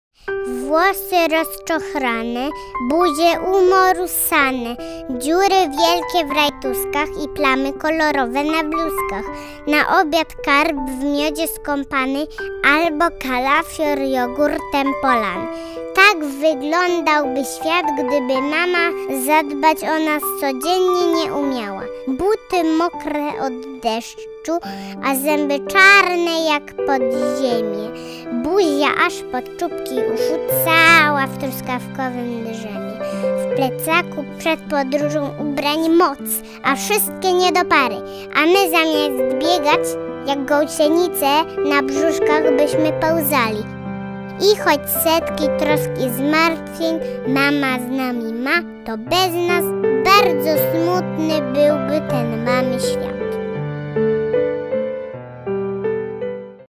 Gdyby nie było mamy
Włosy rozczochrane,
buzie umorusane,
dziury wielkie w rajtuzkach
i plamy kolorowe na bluzkach…

Na obiad karp w miodzie skąpany
albo kalafior jogurtem polany -
tak wyglądał by świat, gdyby mama
zadbać o nas nie umiała.

Buty mokre od deszczu
a zęby czarne jak podziemie,
buzia aż po czubki uszu
cała w truskawkowym dżemie.

W plecaku przed podróżą
ubrań moc, a wszystkie nie do pary
a my zamiast biegać
jak gąsienice na brzuszkach byśmy pełzali.

I choć setki trosk i zmartwień
mama z nami ma
To bez nas
bardzo smutny byłby ten mamy świat​!

Produkcja: limango
Polskie udźwiękowienie: Studio Nomono
Tłumaczenie wiersza: Adriana Pałeczka
Opracowanie polskiej wersji tekstu: Marta Soja